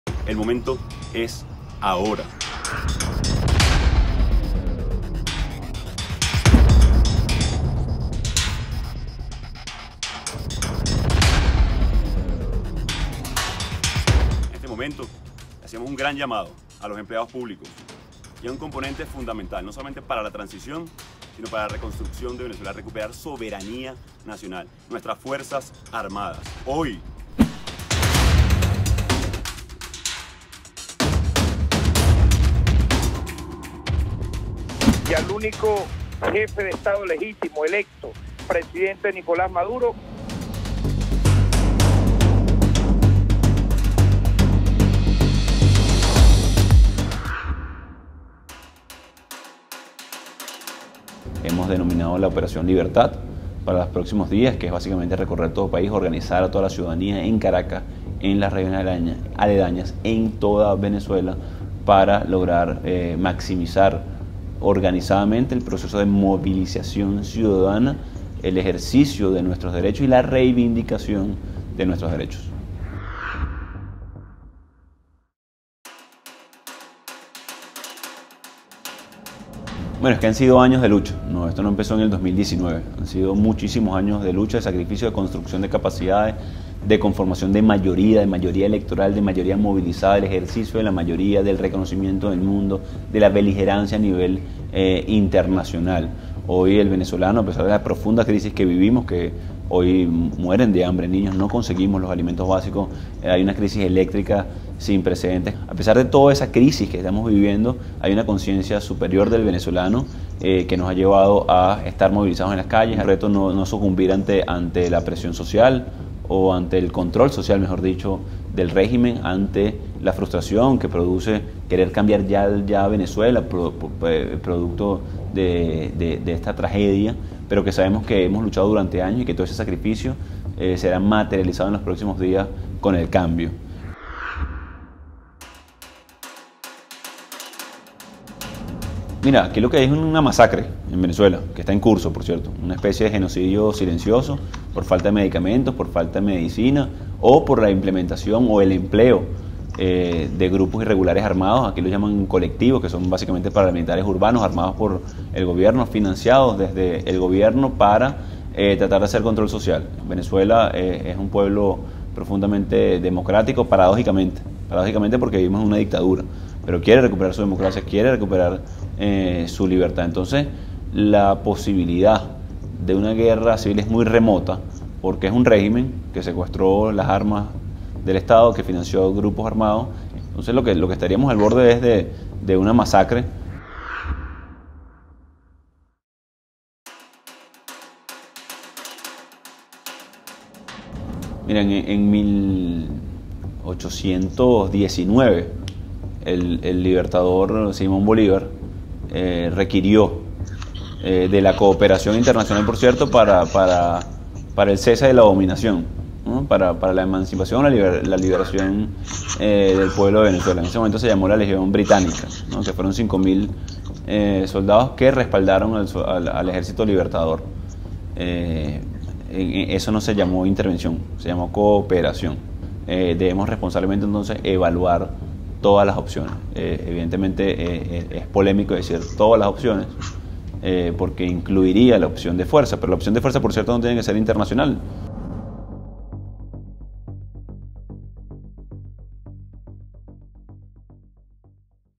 Venezuela's Juan Guaidó leads uprising in Caracas
Venezuela opposition leader Juan Guaidó leads a revolt in Caracas, with limited military support. He spoke with GZERO last month.